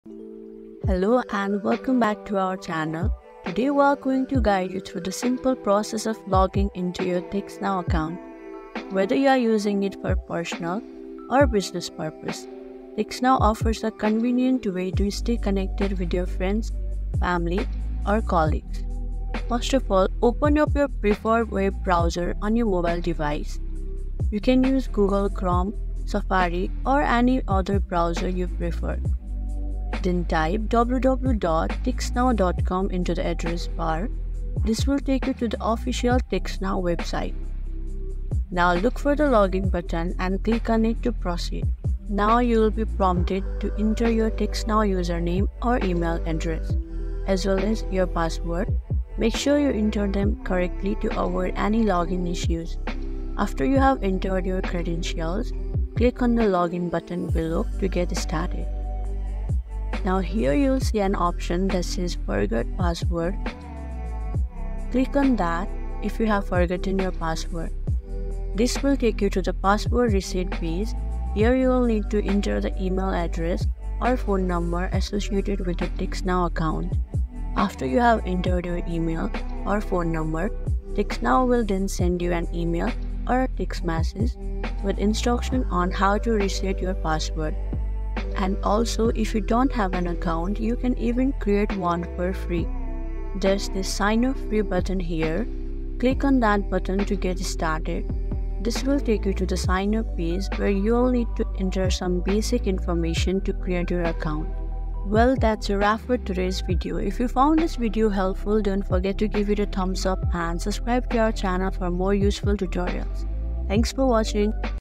How to Login TextNow Account 2024? TextNow Login
Join us for a speedy guide on logging into your TextNow account! Learn the simple steps to access your messages and calls hassle-free. From entering your username to troubleshooting tips, we've got you covered.